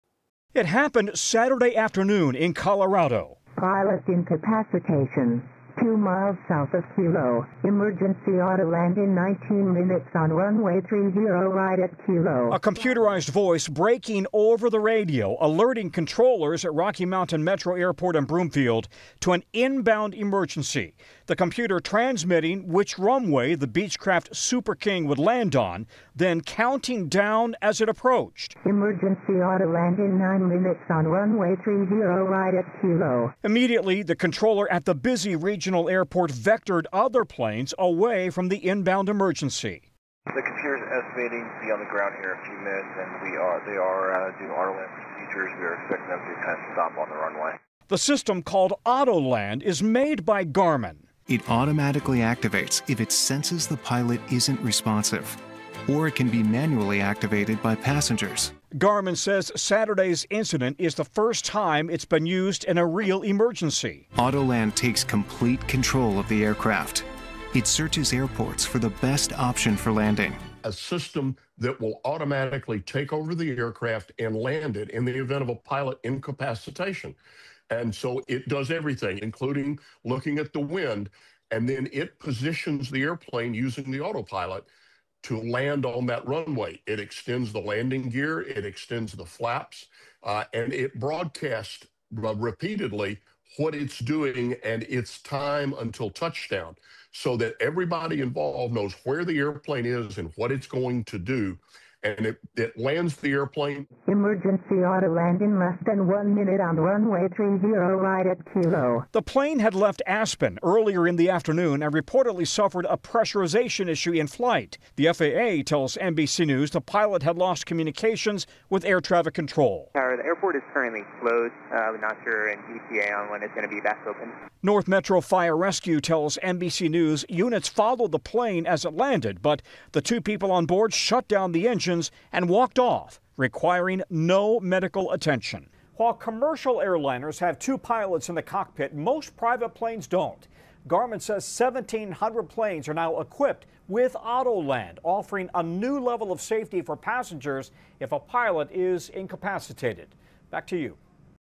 A look at Garmin's Autoland system
About a week ago, a small plane that made a historic emergency landing at the Rocky Mountain Metropolitan Airport in Broomfield. The plane used new technology to safely land on its own.

9NEWS (KUSA) is located in Denver, Colorado.